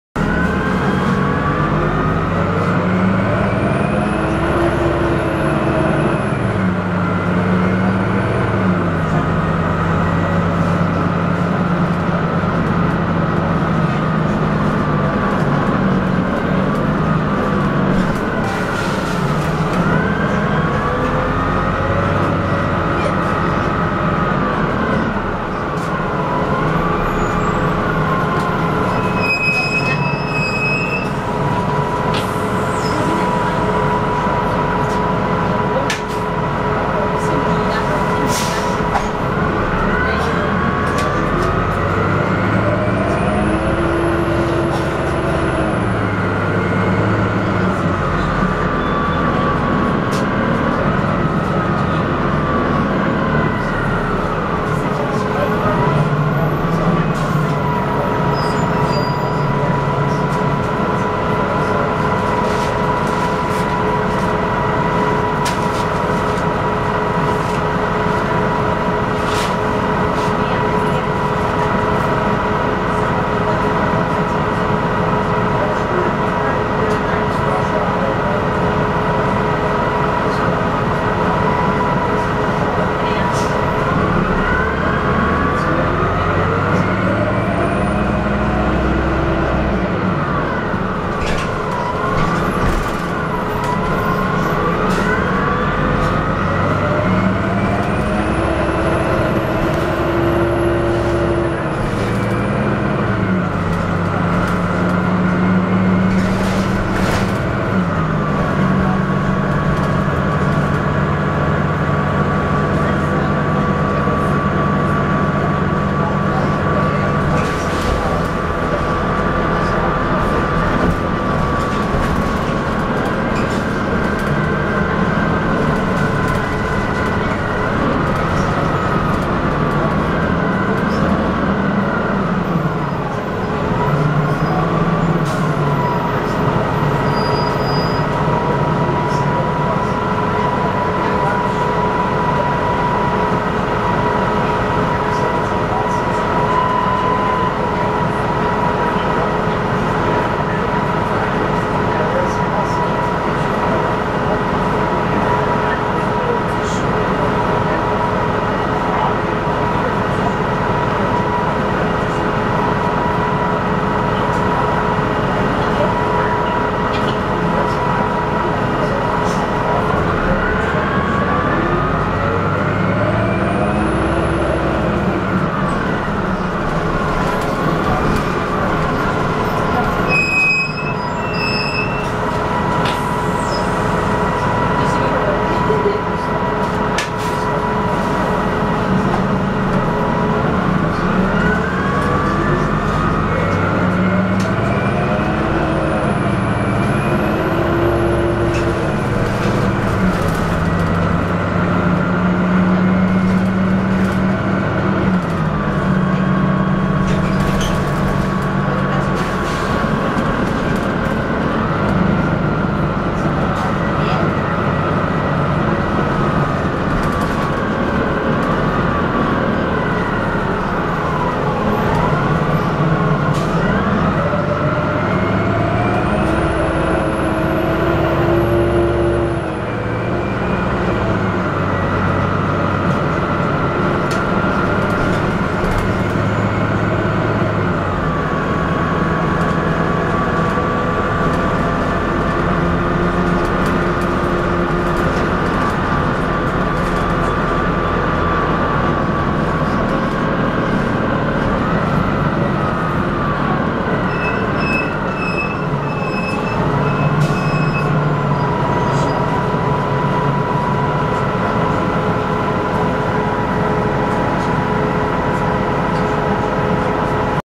New York City Bus #7623 (The Ride)
On Route B11 to Sunset Park.

This is a 2003 Orion VII CNG with a Detroit Diesel S50G-EGR engine and Allison B400R transmission.

NOTE: Notice the funny sound effect made when the bus takes off.